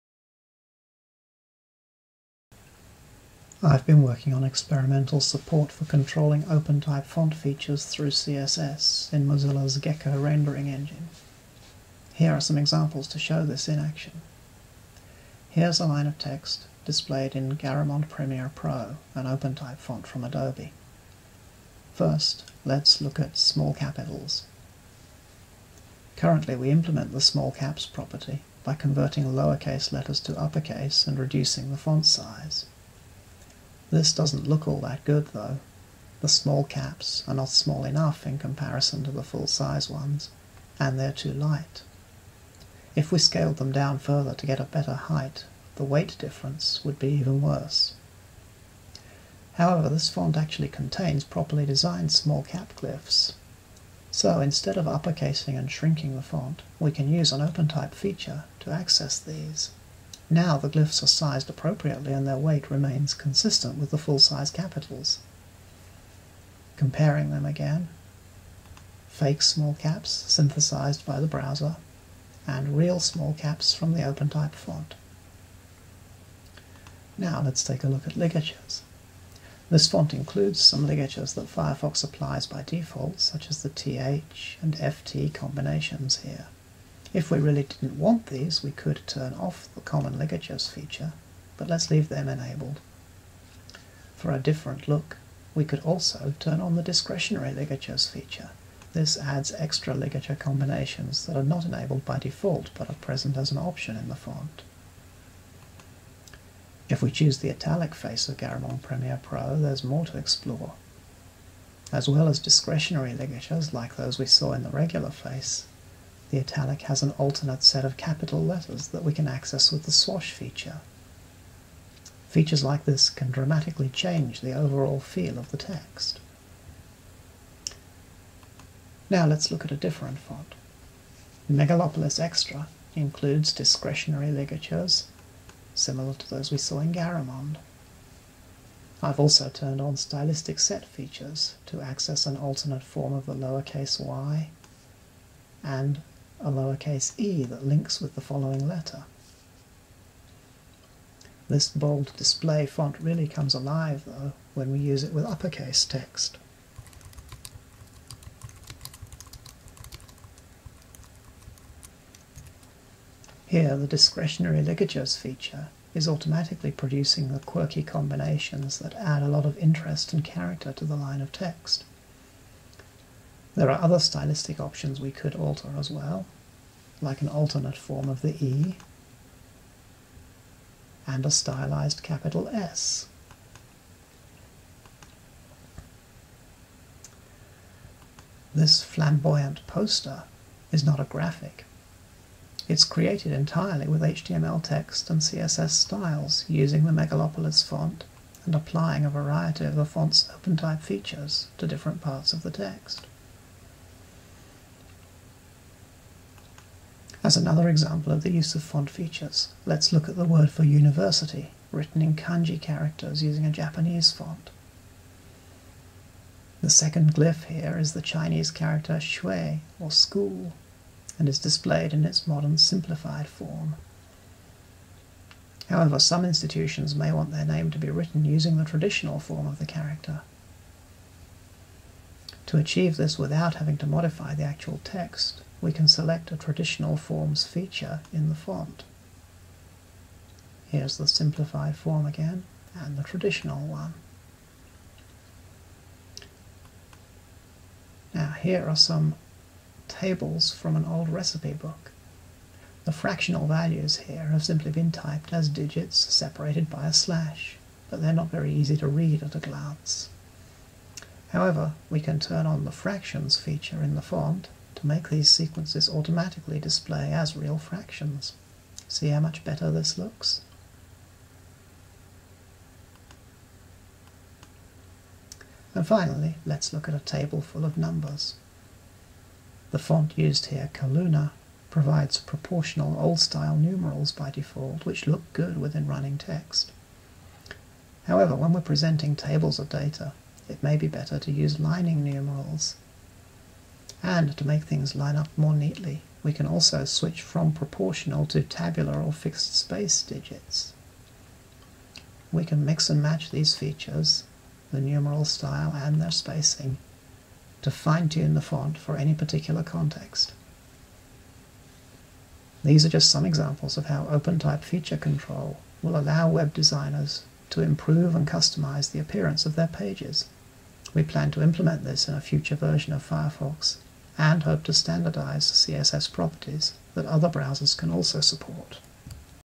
New font control features for designers post Firefox 3.6
This post is from Jonathan Kew and John Daggett. Hes supplied a 5 minute video that shows some of the features on the fly. If youre a total font nerd and you enjoy a soothing British accent, you might want to watch it.